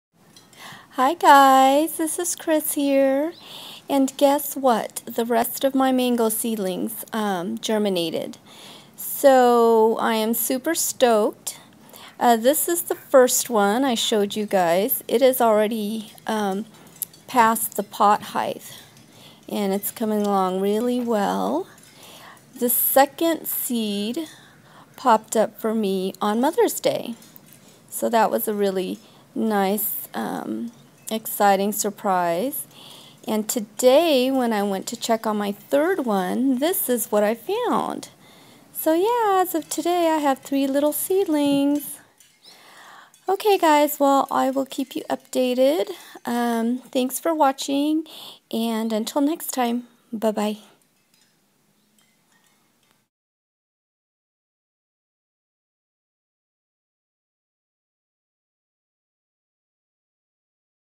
3 lil seedlings | mango trees grown from seeds
yipeee! i am happy to bring you guys an update on my mango trees. the first seedling from last week has grown past the top of the pot, (not the pot height ) .. LOL . the second seed popped up on may 12th and today my 3rd one sprung up out of the soil ... super happy! will keep you all updated in the future : )